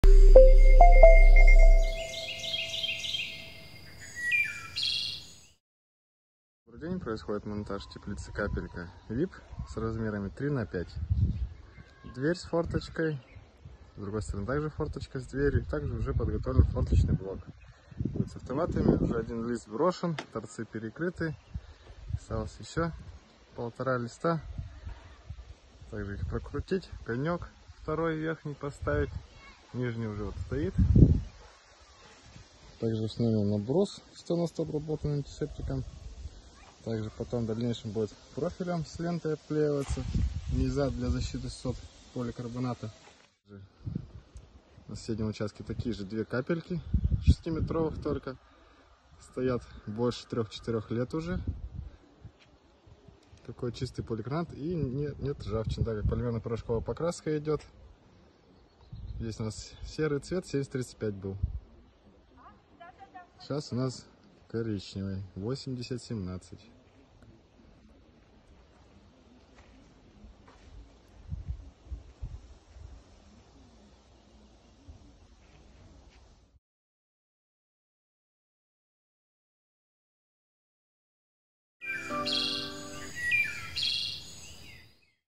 КАПЕЛЬКА 3М ВИП ТЕПЛИЦА КЛАССИЧЕСКОЙ КАПЛЕВИДНОЙ ФОРМЫ С УСИЛЕННЫМИ АРКАМИ
Как избежать проблем зимой?
Скоро посадки.

Вы часто рассказываете мне истории, что теплицы ломаются от снега. Ну что я могу сказать: «Покупайте качественные теплицы у проверенных производителей».
С теплицами компании «Уральская Усадьба»  такого не случается.

Расскажу на примере теплицы «Капелька 3М VIP»:

У этой теплицы каплевидная форма, снег на ней не задерживается.

Но даже если каким-то чудом он налипнет, то ничего не случиться,  потому что у этой теплицы особо усиленный каркас, арки двойные фермы и торцы изготовлены с усилителями из оцинкованной профильной трубы 20х40 и 20/20х1,2 и 1,4 мм. И полимерно порошковой окраской в цвет 8017, шоколад.

Сварка полуавтоматом в среде защитных газов, чистый шов.

Покрыта теплица качественным сотовым поликарбонатом от 4 мм.

Все эти условия позволяют выдерживать снеговые и ветровые нагрузки без ограничений.

Ещё у этой теплицы двойной конек из оцинкованной стали с полимерным покрытием - дождь будет тоже не страшен.

Размеры теплицы «Капелька 3М крепыш»: ширина 3000 мм, длина любая с шагом 1 м. Высота в коньке 2350 мм.

Понравилась!?
Закажите и будьте счастливы!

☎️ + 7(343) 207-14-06
🏡 Екатеринбург ул Климовская 23
🖥 ural-usadba.ru